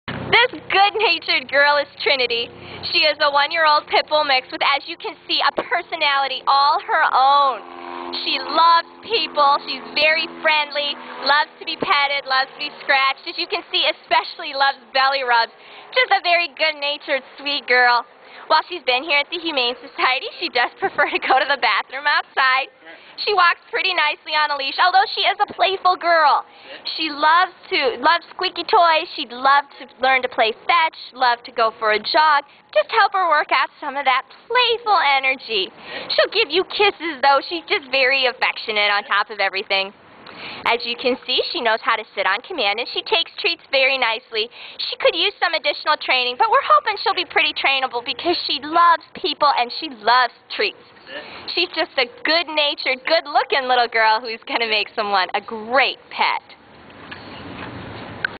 Trinity-Available at the Humane Society of Missouri
TRINITY - ID#A478247

I am a spayed female, chocolate and white Pit Bull Terrier mix.
The shelter staff think I am about 1 year and 5 months old.
I have been at the shelter since Dec 14, 2009.